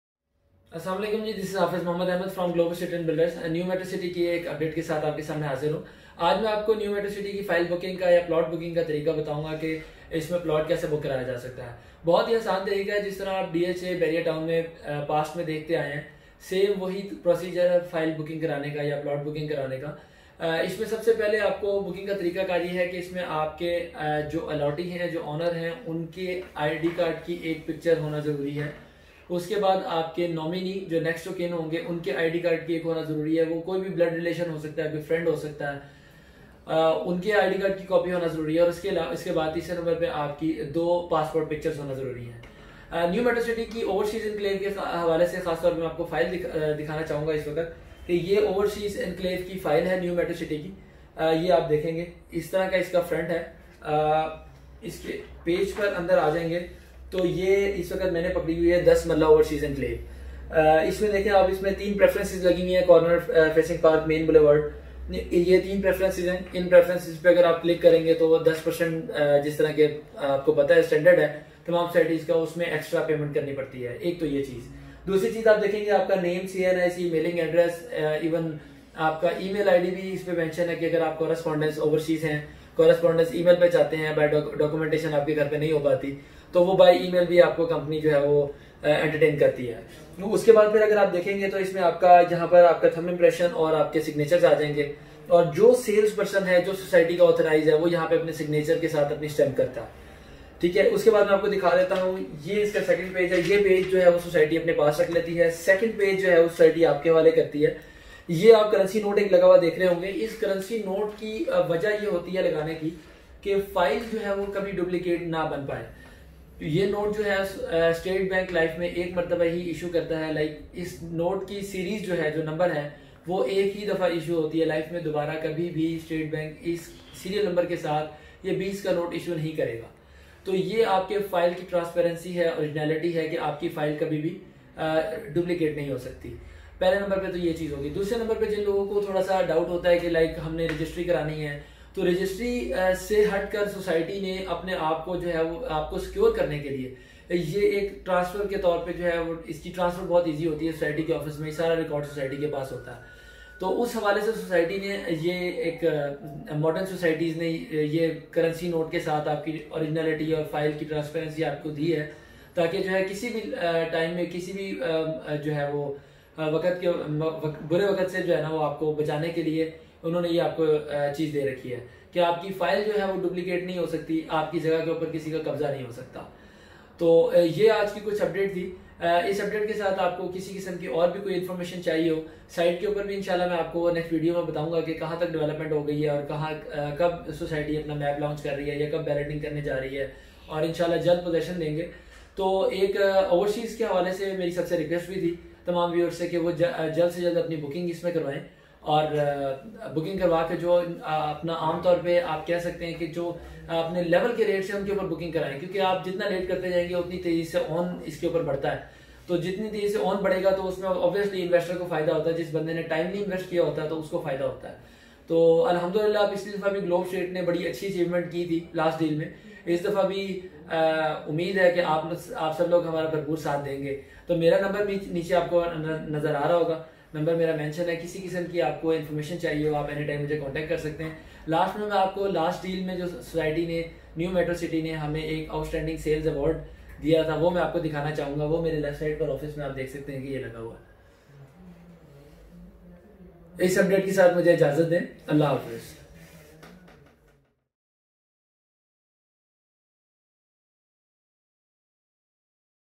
New Metro City Plot Booking Process
How to book a Plot in New Metro City complete detail.New Metro City promises to be the most modern and affordable housing project in Kharian-Sarai Alamgir.